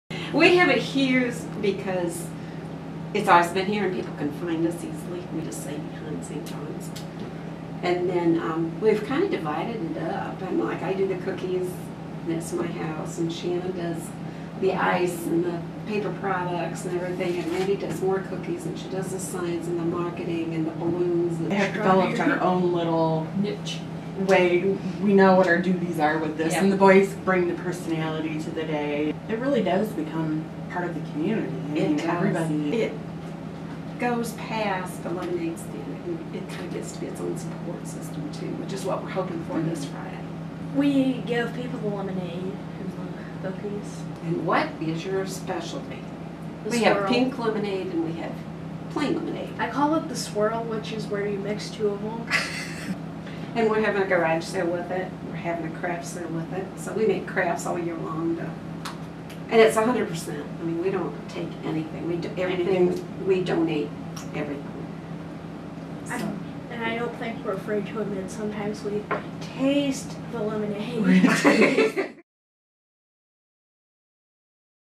Peru brothers' lemonade stand raises money for charity
Collin and Gavin Brown of Peru are hosting their fifth annual lemonade stand for charity. This year, the proceeds will go to the Pink Ribbon Club, a breast cancer support group. Also featured in the video are lemonade stand helpers Maxine McQueen, the boys grandmother, Shanon Edwards, their godmother, and their mom, Randi Brown.